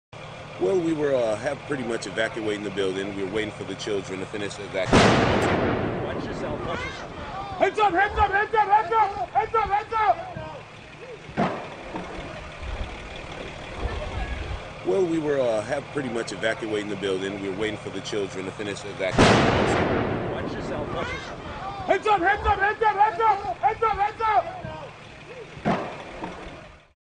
Manhole Cover EXPLODES On Live TV!!!
A TV news crew was filming as an underground explosion sent a manhole soaring up into the air causing panic as the heavy object fell from sky.

The manhole, which could be as heavy as 50kg, comes down to the pavement with a loud crash close to a man who appears to be an emergency worker. The explosion was the second in a series of three that rocked the busy street and came after several nearby buildings had been evacuated. Power was cut off to the area and the fire brought under control.

The cause of the fire is yet to be determined but it’s believed the explosions were probably caused by a buildup of heat, gas and smoke in the confined space underneath the road.

Video Credit: WGRZ